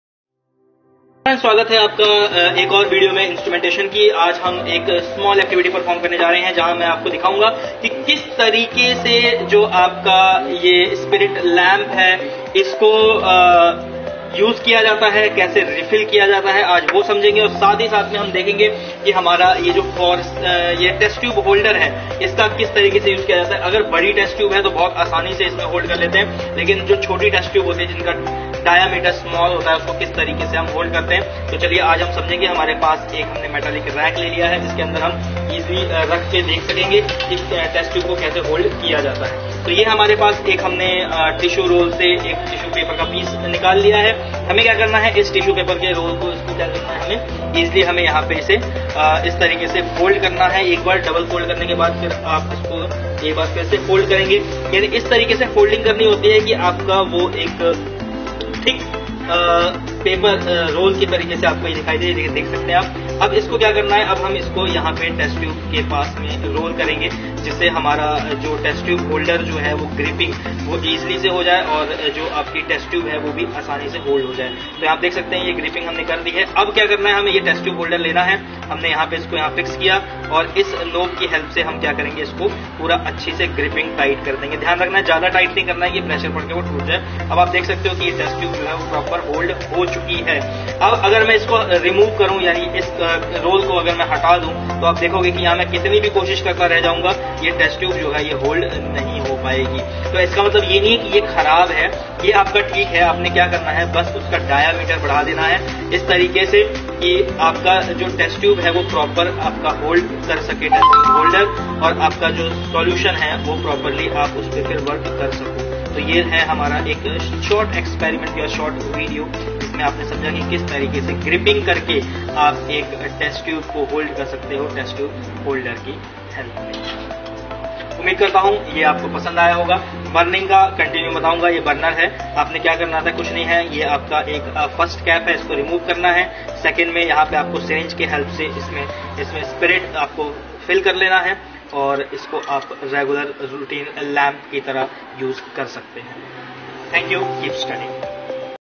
Test tube Holding With the help of Test tube holder
A test tube rack, sometimes called a test tube stand, ranges in color, size composition, and material used in manufacture. Test tube racks are made to hold multiple test tubes upright at one time for easy organization, transport, or washing, or for placement in an autoclave or refrigerator.

A test tube holder is used to hold test tubes. It is used for holding a test tube in place when the tube is hot or should not be touched. For example, a test tube holder can be used to hold a test tube while it is being heated.

It is a common piece of laboratory apparatus used to hold various test tubes and other apparatus. it is also known as a clamp and has a general length of 6.25 inches long with finger grips. it can hold any kind of test tube being hot and untouchable

Beaker tongs are used to hold and move beakers containing hot liquids.